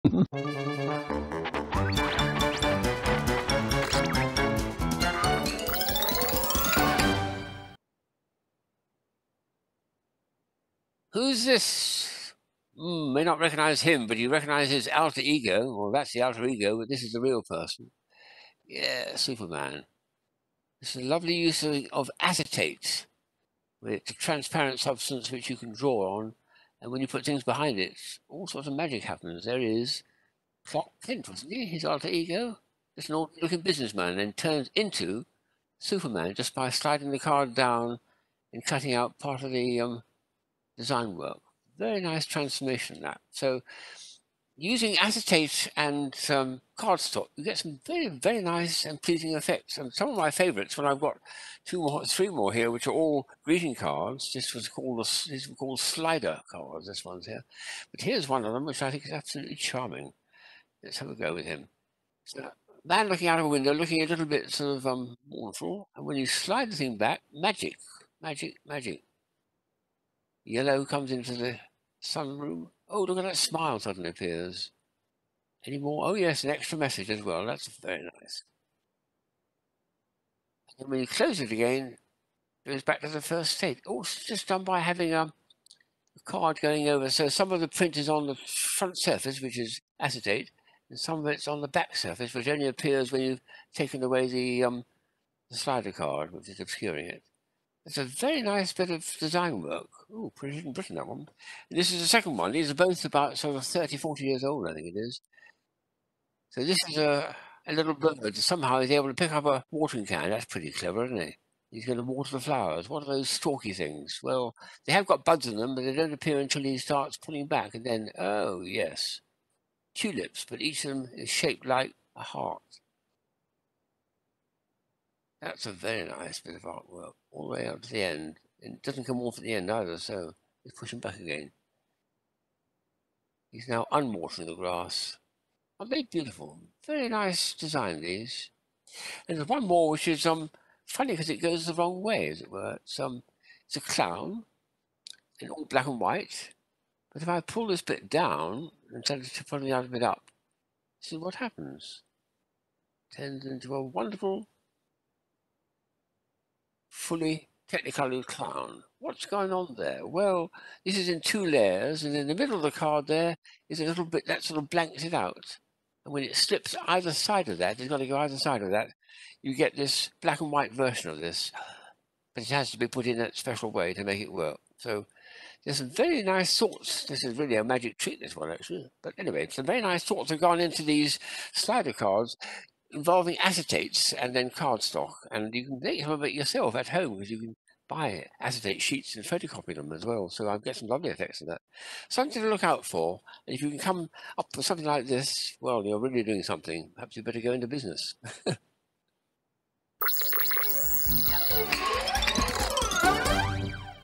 Now You See It...
Now You See It... Now You Don't...

A little card showing a figure of a man. Who is it? Well, slide the little card in, and we see... Superman! The man was Clark Kent all along! This little card uses acetates in order to be able to reveal and hide different parts of the picture.

Tim then shows a 'Slider' card. A man is looking out of the window looking a little sad. Slide the central section out, and the window becomes flooded with sunlight, the man smiles, and a message appears.

The next card is from the same company, and shows a little bird flying along, carrying a watering can. As you slide out the insert, all the little stalks becomes flowers. These are really special greeting cards! Tim thinks they are around 40 years old.

Finally, a black and white clown. Pull out the central section, and the clown appears in full colour.

As Tim says, if you have some card and some acetate sheets, there are things you can even make something like this at home!